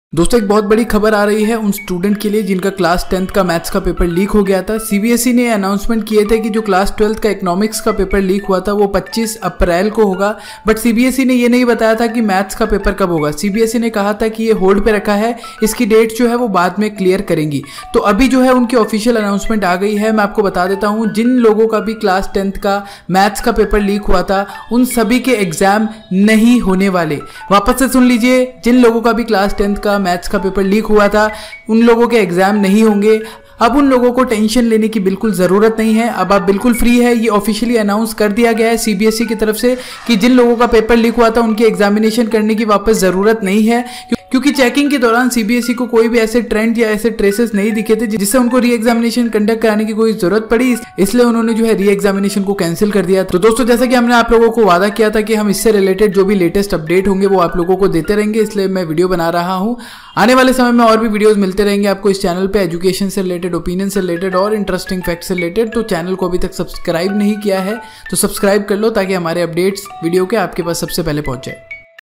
CBSE Class 10 Maths Re- Exam Official Announcement | 3rd April 2018 | Latest Update |CBSE Paper Leak
CBSE Class 10 Maths Re- Exam Official Announcement | 4th April 2018 | Latest Update |CBSE Paper Leak